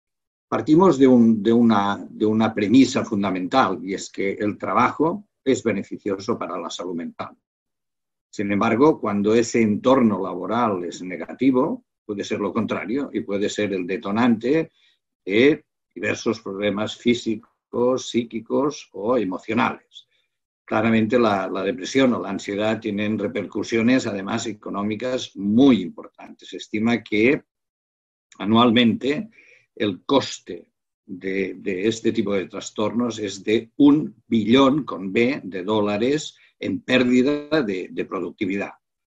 Luis San Molina
Luis San, psiquiatra consultor senior en Parc Sanitari Sant Joan de Déu, ha revisado algunas de las cifras relacionadas con la salud mental en el lugar de trabajo tanto en España como en Europa. Según los datos aportados por el experto psiquiatra, 1 de cada 10 trabajadores cree que no gestiona bien el estrés en el lugar de trabajo. San se muestra convencido que las organizaciones pueden y deben trabajar en la promoción de la salud y la salud mental. «Se trata de un fenómeno de gran relevancia», ha explicado, «en Europa, los trastornos mentales son el segundo problema más importante en el ámbito del trabajo».